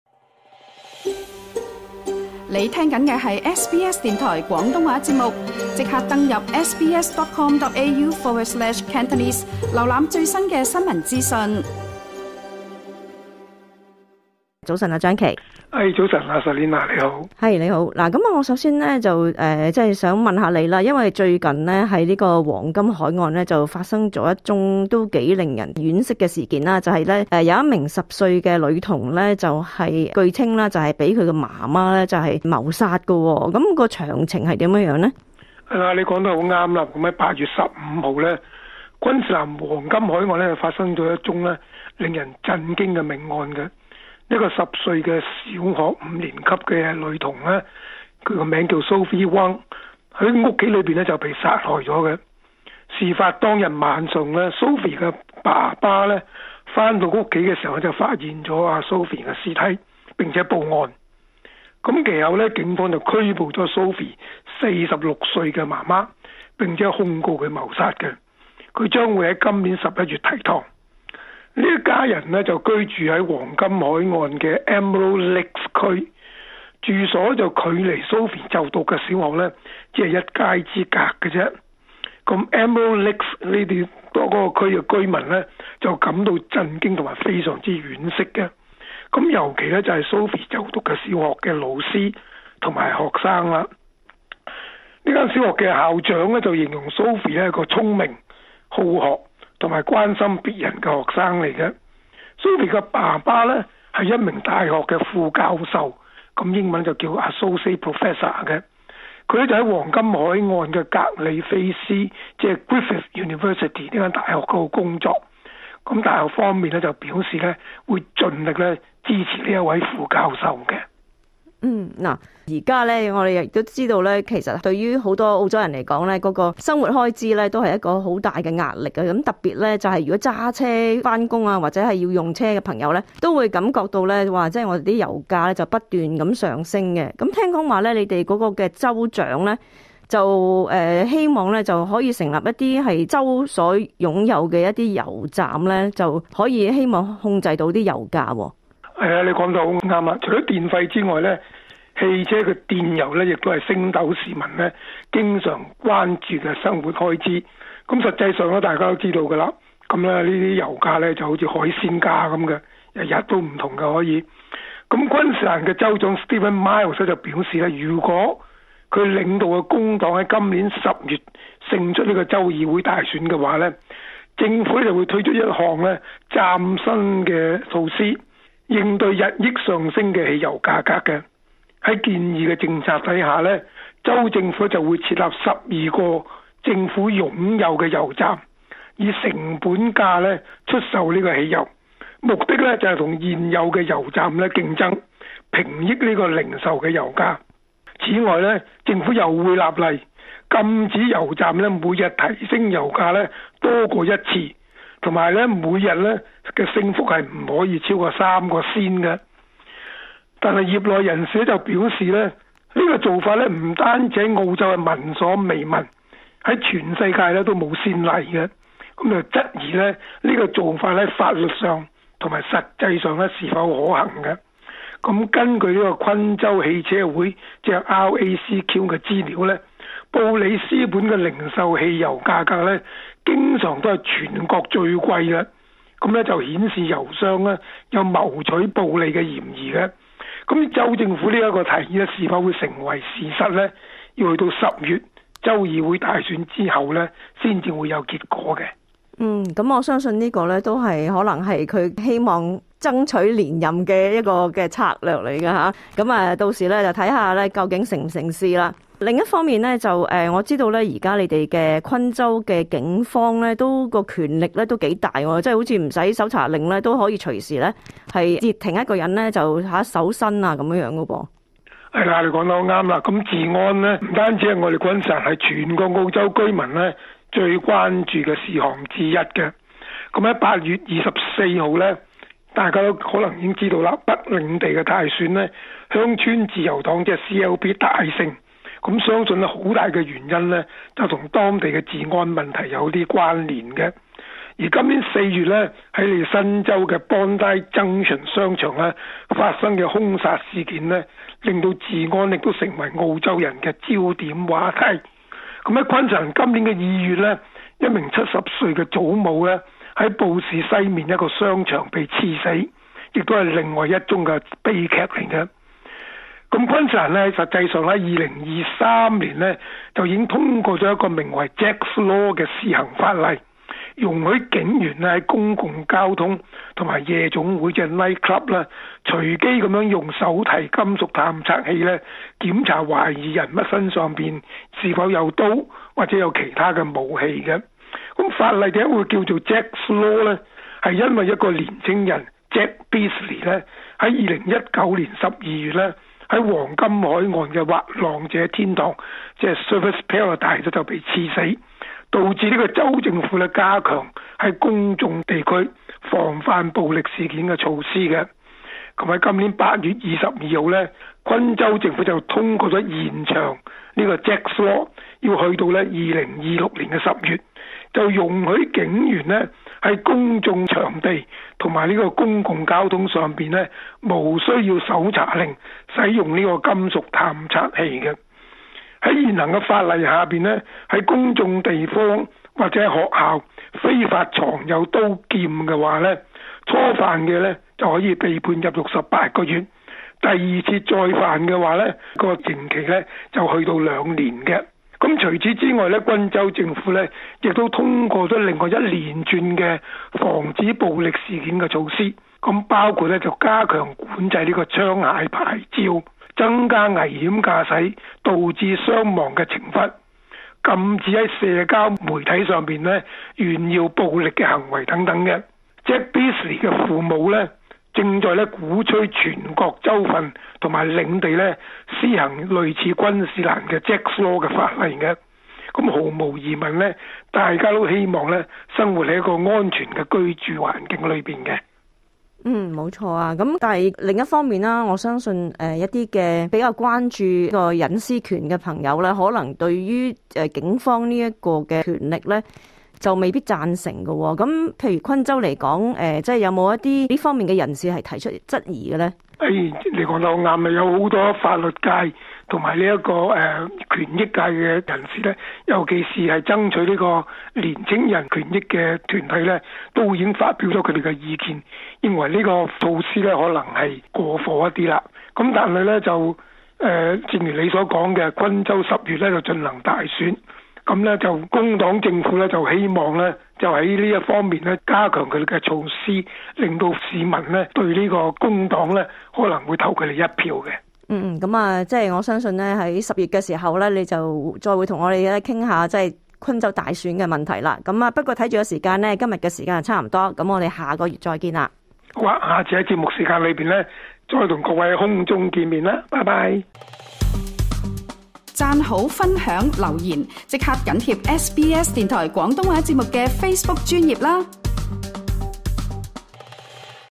【與民爭鋒？】昆州州長承諾如果連任會設立政府油站以成本價出售燃油 | SBS Cantonese - SBS廣東話節目
昆州州長承諾，如果在10月舉行的州政府選舉中再次當選，就會設立由州政府擁有的油站，以成本價出售燃油